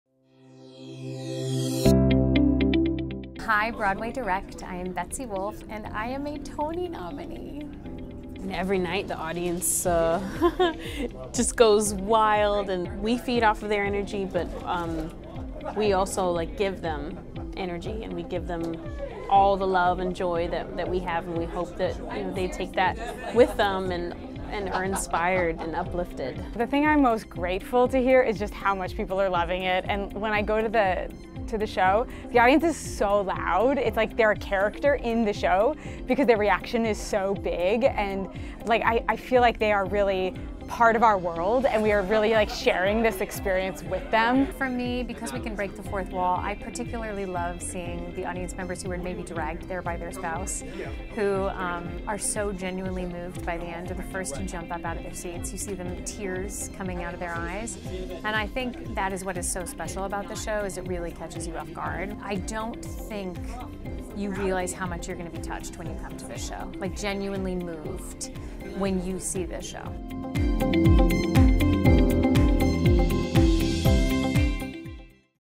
& Juliet Tony Nominees on the Joy of Breaking the Fourth Wall
Broadway Direct sat down with 2023 Tony Award nominees Lorna Courtney, Jennifer Weber, and Betsy Wolfe to chat about how breaking the fourth wall at & Juliet enhances the audience response.